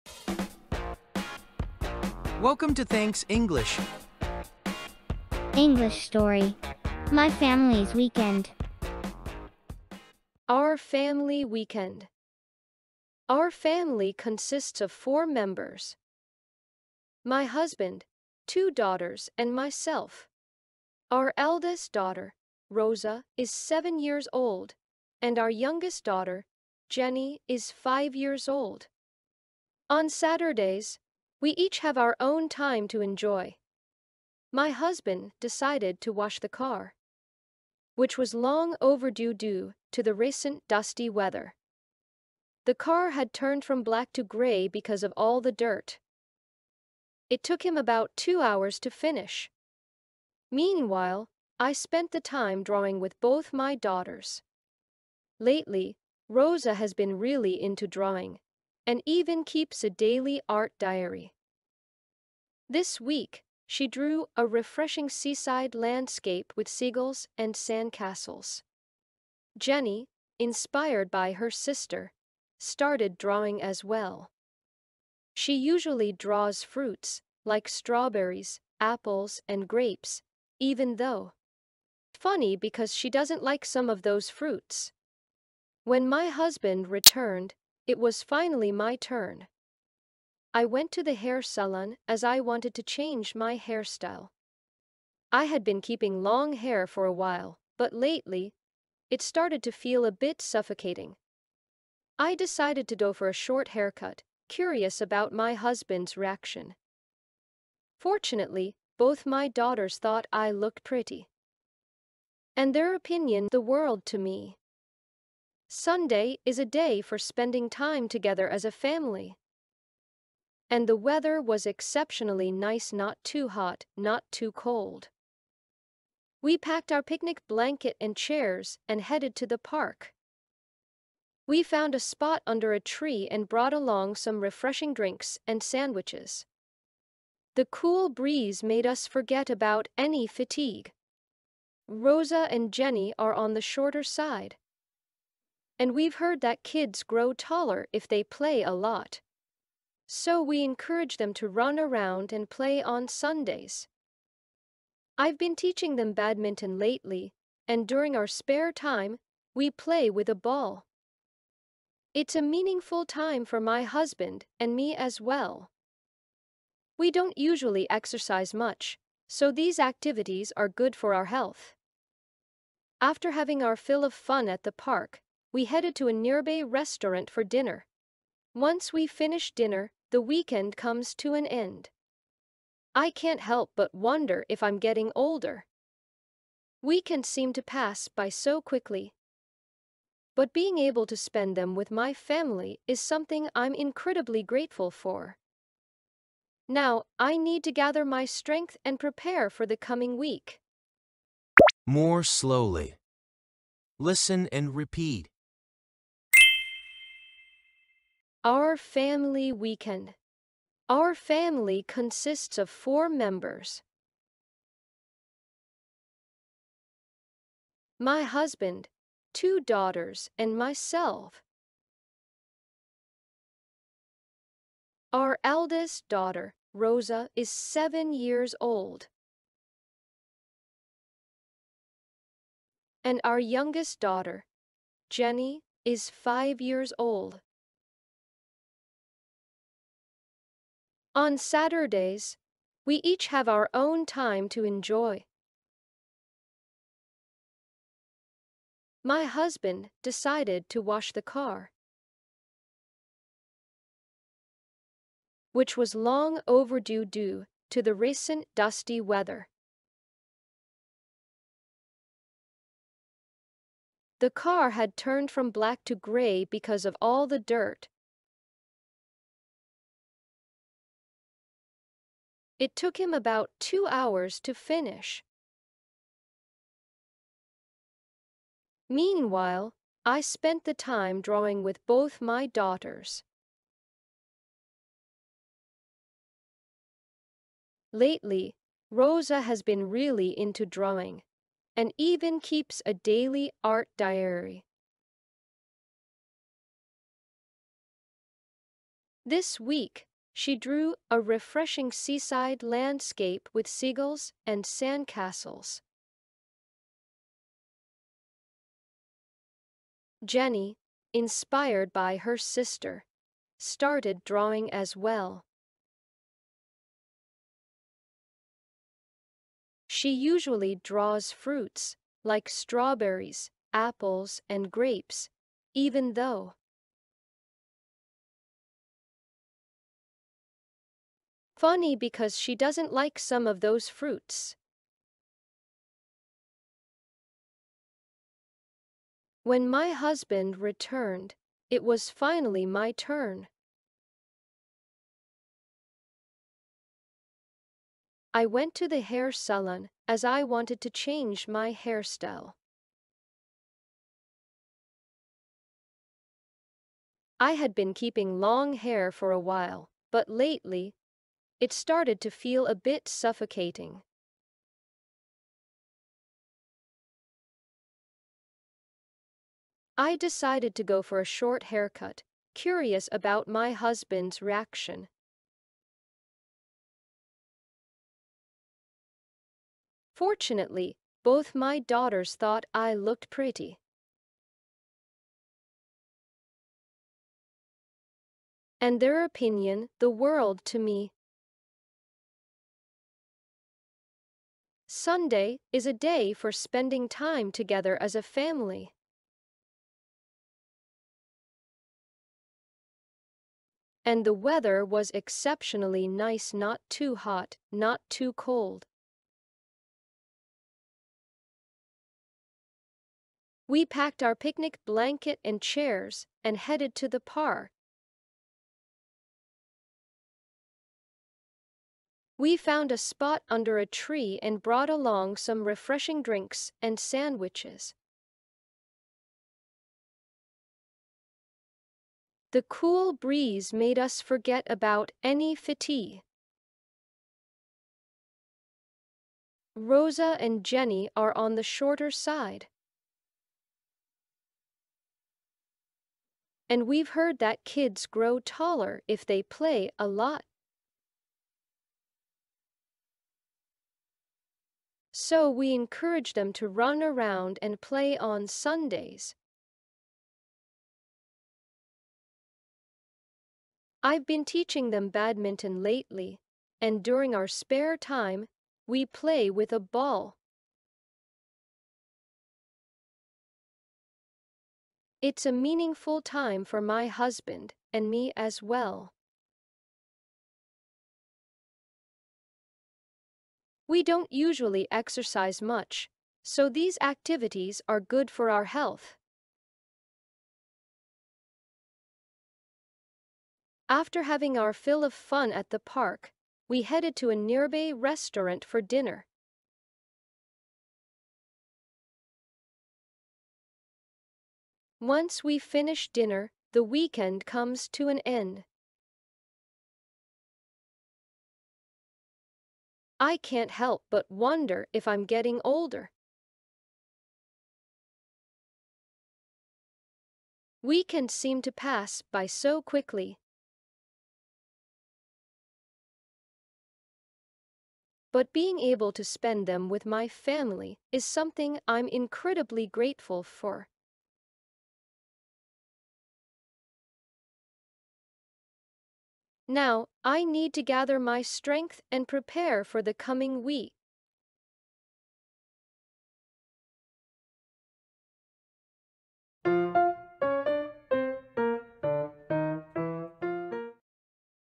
English story for beginners-weekend/Listen and repeat/English practice/improve your English
Thanks English: weekend

Use according to the situation.
This story organizes situations and sentences
 that are often used in daily life,
which can help improve beginner's English skills.
Especially, it sparks interest in English with interesting stories.
By repeating them three times,
English will flow effortlessly from your mouth!!
If you want to learn English with a new video,